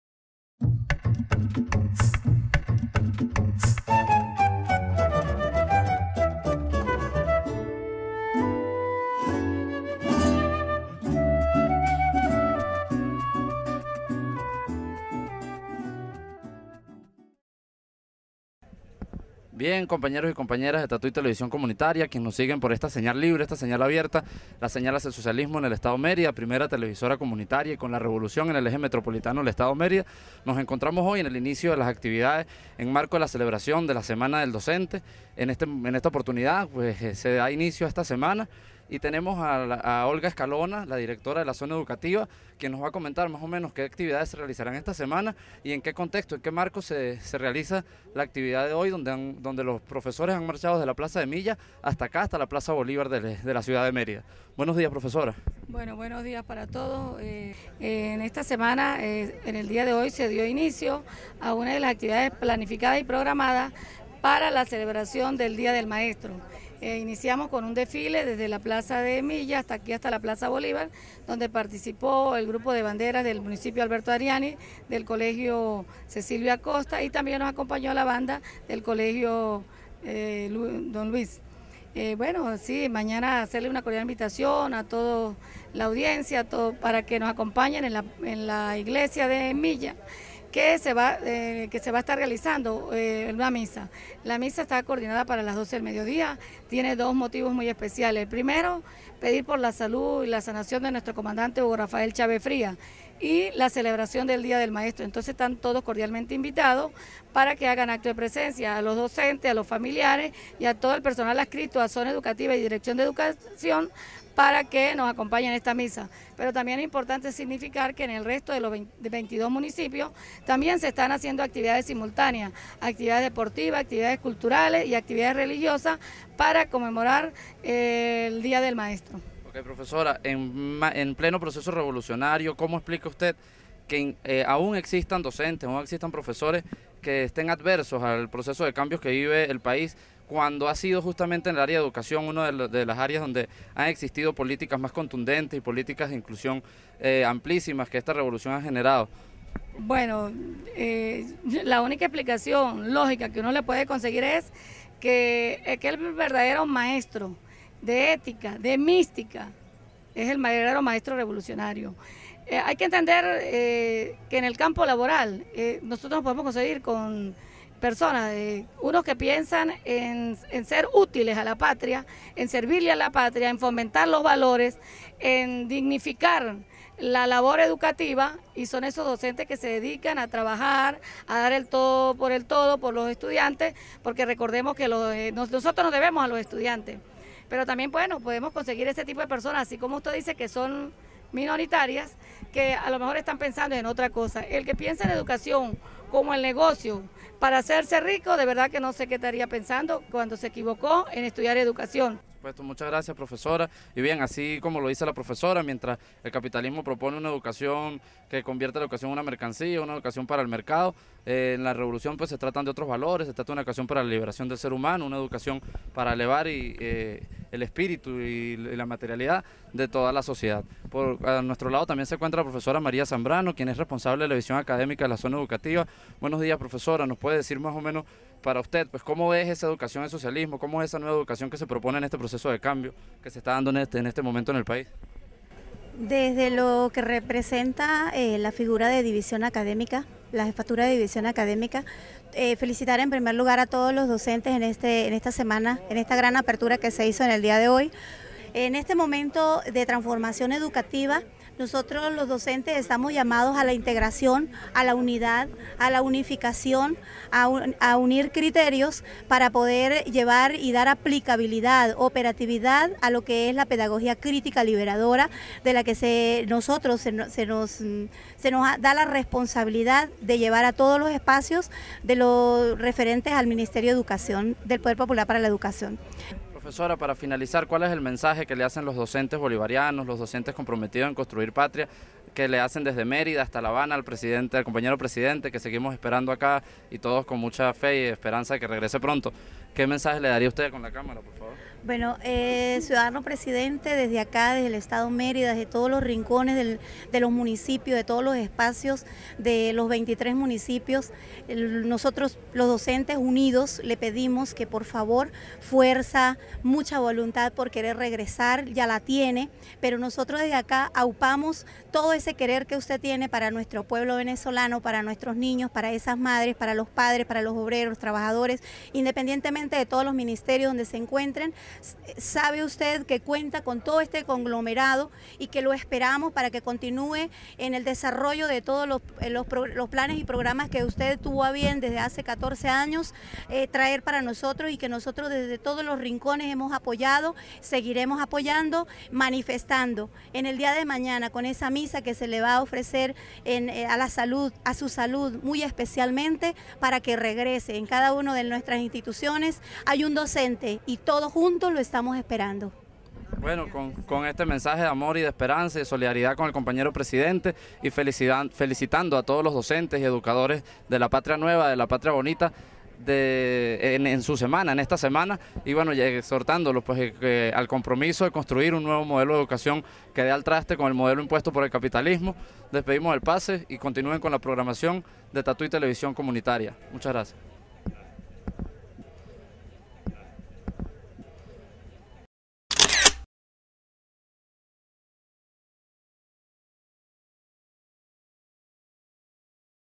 Aquí Tatuy Informa: Desfile de Educadores en Solidaridad con Chávez
Este lunes 14 de enero, los educadores y educadoras del estado Mérida, iniciaron sus actividades conmemorativas en el marco de la semana del educador, esta vez desfilando, junto a las bandas de dos importantes liceos del estado, desde la plaza de Milla hasta la plaza Bolívar de la ciudad capital. Docentes y directivos de la zona educativa hicieron particular énfasis en la invitación a la misa del 15 de enero en oración por la recuperación del Comandante Chávez, llamando además a estar atentos a las distintas actividades deportivas y culturales que se desarrollarán esta semana en los 23 municipios del estado.

La Profesora Olga escalona, directora de la Zona Educativa, comenta que el verdadero maestro de ética y de mística, es el Maestro Revolucionario que piensa en fomentar los valores y ser útiles a la patria. Sin embargo hay otros pocos que solo los mueve el interés de su ambición material, a estos últimos, les exhorta que se sumen al trabajo que impulsa el gremio con amor y vocación mas allá de las reivindicaciones, dentro de las cuales, La profesora informó que el pago pendiente con los docentes se irá regularizando esta misma semana.

Por otra parte la profesora María Zambrano envió un mensaje emotivo, de apoyo al líder de la Revolución, el Comandante Hugo Chávez, pidiendo por su pronta recuperación física, para que siga guiando el curso de éste proceso de cambios hacia la construcción del Socialismo.